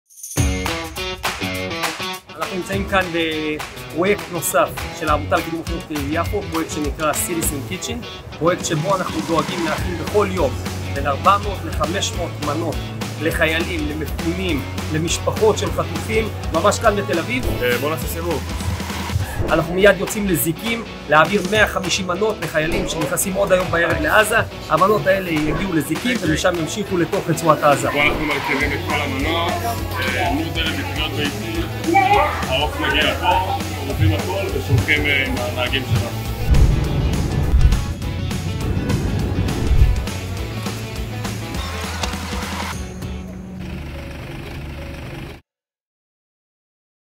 Citizens Kitchen in partnership with the Jaffa Institute
Dear partners,

More than 12 weeks have passed since October 7th.

We are going through difficult days as a society.

We at the Jaffa Institute are doing everything in our power to make it as easy as possible for the soldiers, the evacuees, the families of the abductees, the bereaved families and the thousands of regular participants in our programs.

Here is a short video of an additional project, in which the Jaffa Institue is a partner, called "Citizen Kitchen," which serves hundreds of meals to families of hostages, soldiers and evacuees every day.

Citizen Kitchen was founded by:

Alia Pasteman
Shaindell Davis
Zack Javitt (who initiated the connection with the Jaffa Institute) and
Alon Sharabi

Hoping for better days!